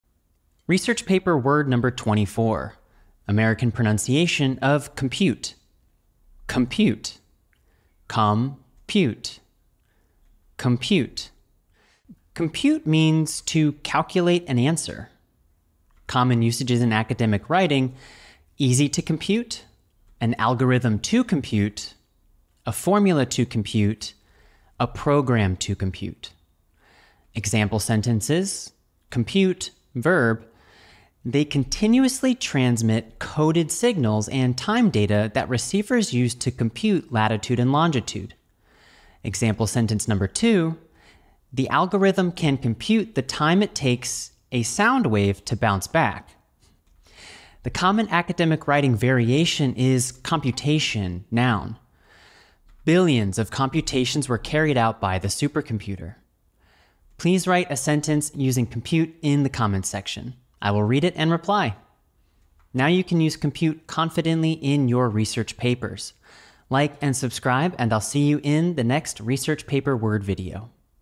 COMPUTE definition, COMPUTE in a sentence, COMPUTE pronunciation | research paper word #24
COMPUTE definition, COMPUTE in a sentence, COMPUTE meaning , COMPUTE pronunciation. Use the noun COMPUTE correctly in research and academic writing.

CHAPTERS

0:00 Introduction to COMPUTE
0:06 COMPUTE pronunciation
0:12 COMPUTE definition, COMPUTE meaning
0:17 COMPUTE usage in academic writing
0:27 COMPUTE in a sentence, COMPUTE example sentences

SCRIPT OF VIDEO

Research paper word number 24

American pronunciation of compute:
compute
com-pute
compute

Compute means: to calculate an answer

Common usages in academic writing:
easy to compute
difficult to compute
an algorithm to compute
a formula to compute
a program to compute

Example sentences, compute (verb): They continuously transmit coded signals and time data that receivers use to compute latitude and longitude.

Example sentence number 2: The algorithm can compute the time it takes a sound wave to bounce back.

The common academic writing variation is computation (noun): Trillions of computations were carried out by the supercomputer.

Please write a sentence using “compute” in the comments section. I will read it and reply.

Now you can use “compute” confidently in your research papers.

UNI-EDIT SERVICES FOR AUTHORS

Uni-edit specializes in language services for academics and researchers.

REW019